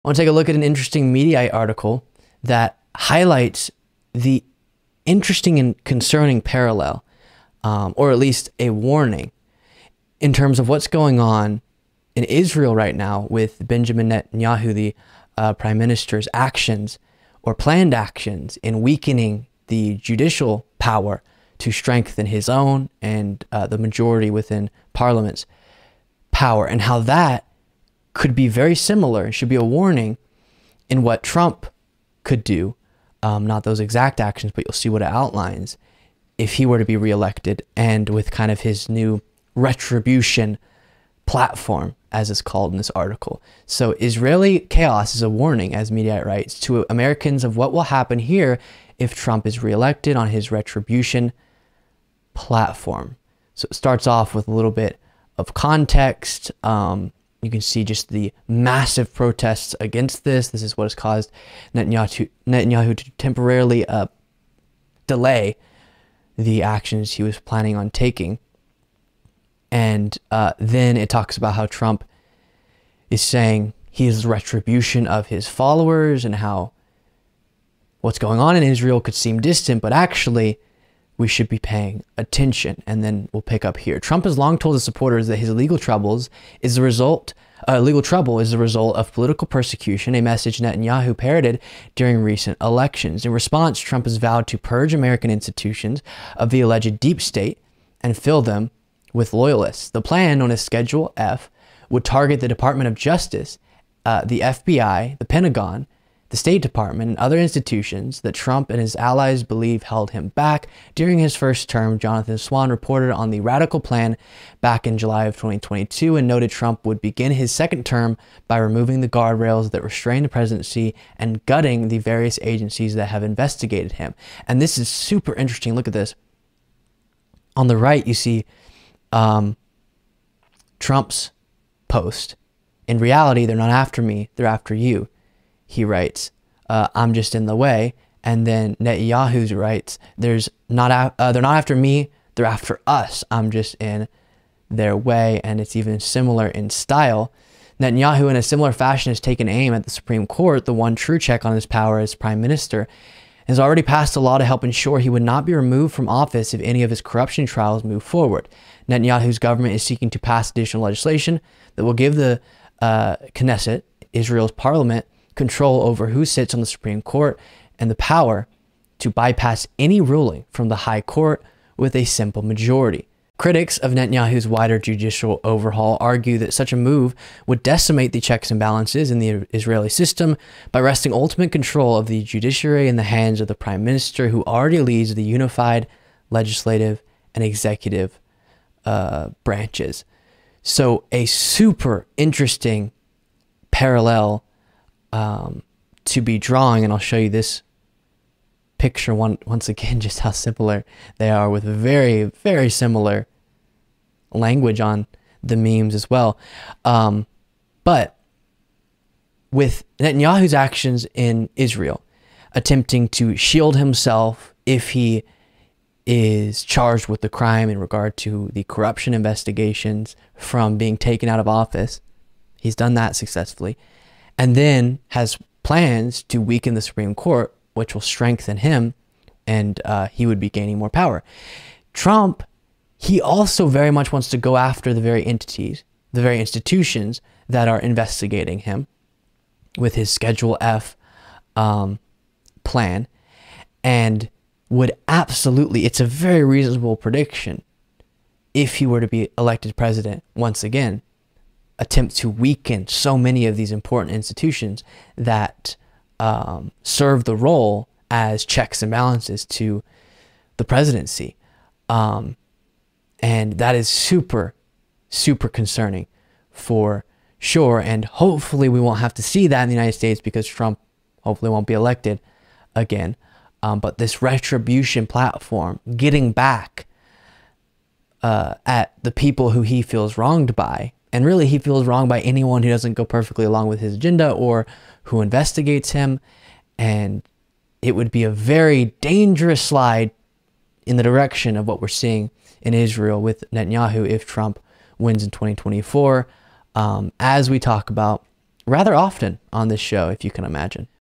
this is concerning
The parallel between the current actions being taken by Israeli Prime Minister Netanyahu and what a possible second Trump presidency could look like is concerning.